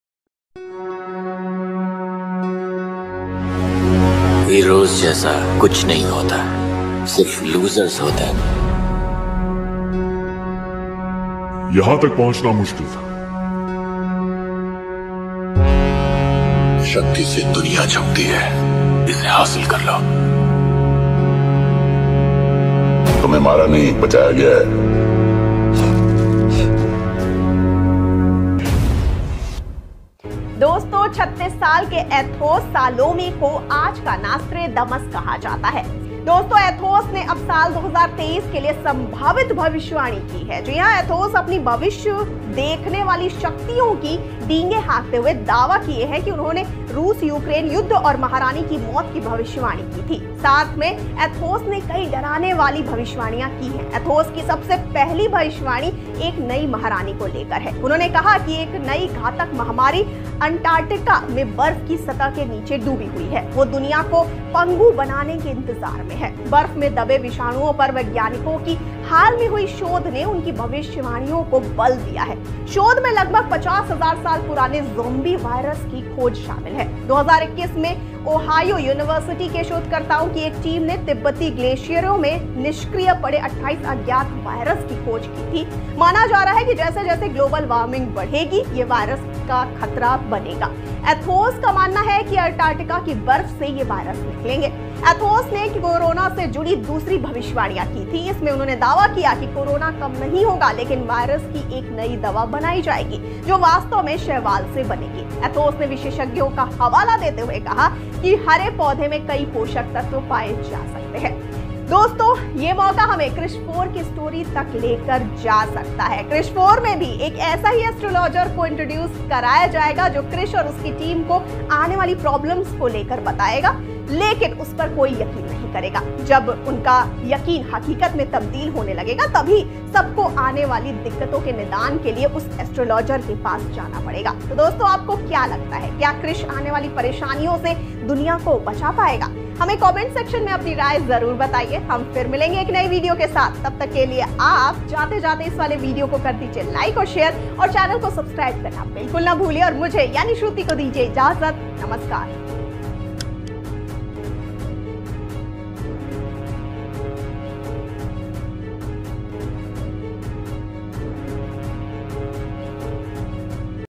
Krrish 4 | कौन है Aethos !! | Hrithik Roshan | Deepika Padukone | Priyanka Chopra | Rakesh Roshan
Hrithik Roshan, who tasted success in 2019 with Super 30 and War, is now gearing up for Krrish 4 that would reportedly bring back the alien Jaadu after 16 years.

KEY QUESTIONS-
0:00 Intro
0:32 Kya Kaha jaata hai Aaj k Athos ko ?
0:35 Kya Bhavishyavaadi ki hai Athos n ?
0:57 Kisko lekar hai Pehli Bhavishyavaadi Athos ki ?

Filmmaker Rakesh Roshan gave Bollywood probably the first superhero franchise – Krrish. Starring his son and actor Hrithik Roshan, the franchise kicked off in 2003 with Koi Mil Gaya. The origin story of the titular character was presented in Krrish (2006), with Krrish 3 (2013) being the last release.

Hrithik Roshan is known for choosing films wisely. Lately, even though the actor has appeared in very less films, all of them have been highly successful. But the biggest of all is yet to come as expectations are extremely high from the fourth instalment of HR’s superhero franchise. In order to meet them, director Rakesh Roshan is developing a water-tight script.

As per the report in Pinkvilla, Rakesh Roshan is working on the important part of Krrish 4’s script. Once it’s done, the casting and other things will kick off. “The fourth instalment of the Krrish franchise will take the story of the third part ahead, but it will be set in a completely different world with new characters and exciting twists. Rakesh ji is developing the script and is presently working on the crucial parts of the story. Once he finalises the script, they will begin work on the casting and other logistics of this massive project. It will also feature some never seen before action sequences,” the source added.

Some earlier reports had stated that Krrish 4 will be Hrithik Roshan’s most expensive film ever and it will be mounted on superlative VFX stuff. And now with such an interesting update coming in, expectations are really high whenever the film hit the shores.

Description
Krissh4 is an upcoming sci-fi fictional film and 4th sequel of the franchise film of Krishh. Starring Hrithik Roshan and priyanka chopra in lead role. It is one of the highly anticipated fictional film and has been expected to come off since it’s 3 part in 2013

Thumbnail: कौन है Aethos?

A- Shruti Dubey
W- Manisha Jain
E- Ankush Thakre